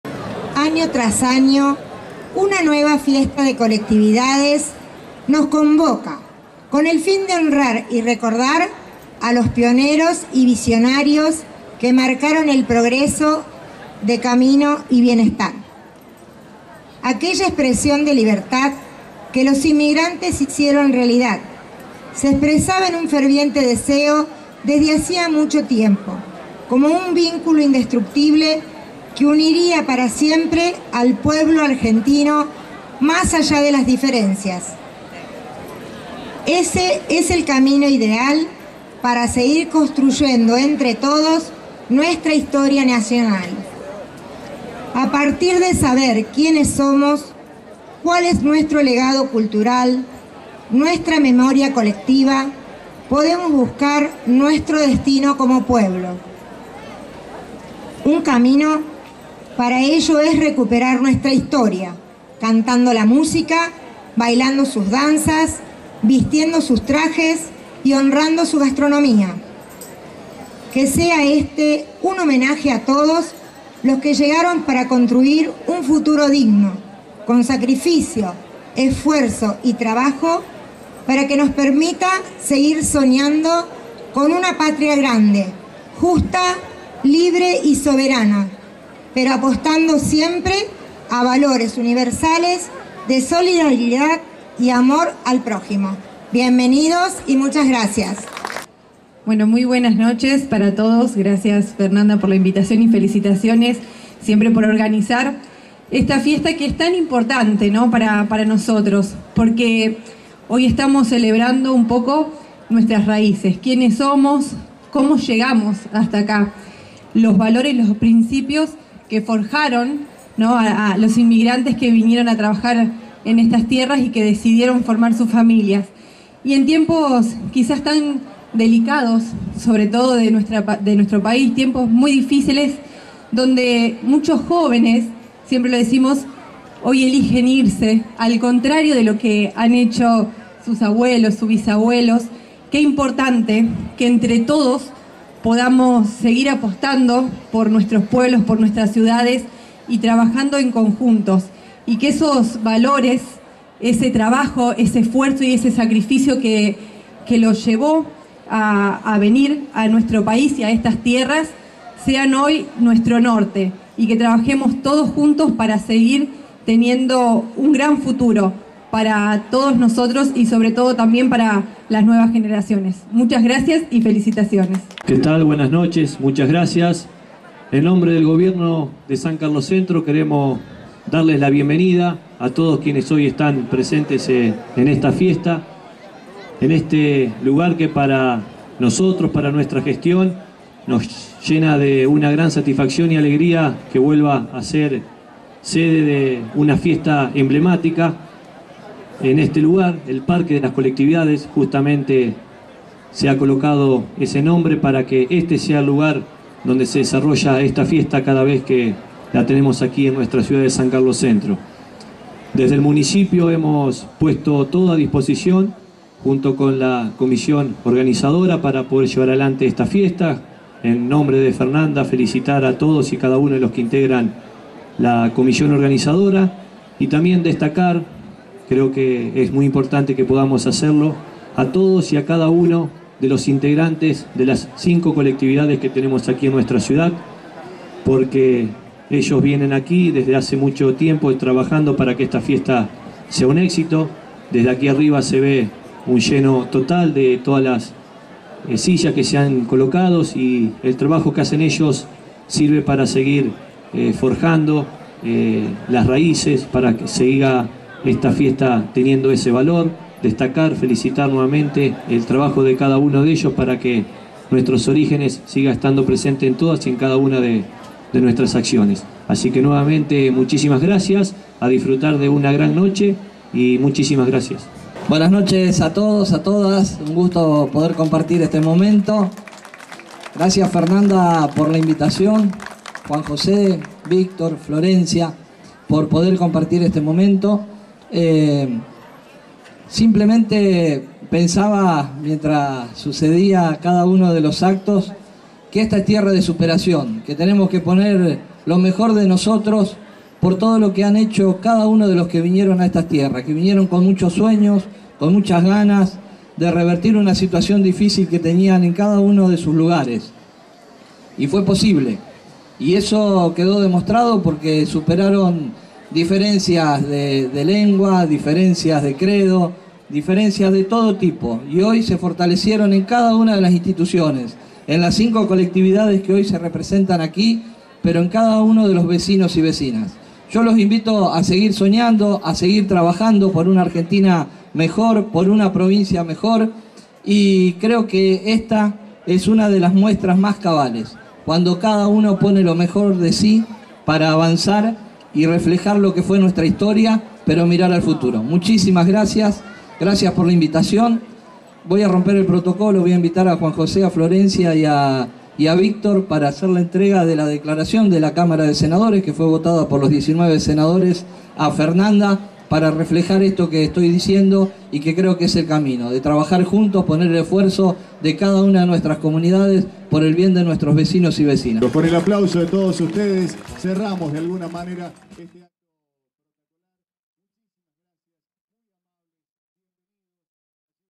ACTO APERTURA 25º EDICIÓN DE LA “FIESTA DE LAS COLECTIVIDADES - SAN CARLOS CENTRO
SAN CARLOS CENTRO
MA. FERNANDA CORNIER - PTA. COMISIÓN FIESTA DE LAS COLECTIVIDADES
JIMENA SENN - DIPUTADA PROVINCIAL
RUBÉN PIROLA - SENADOR PROVINCIAL
J.J.PLACENZOTTI - INTENDENTE
ACTO APERTURA 25º EDICIÓN DE LA “FIESTA DE LAS COLECTIVIDADES”.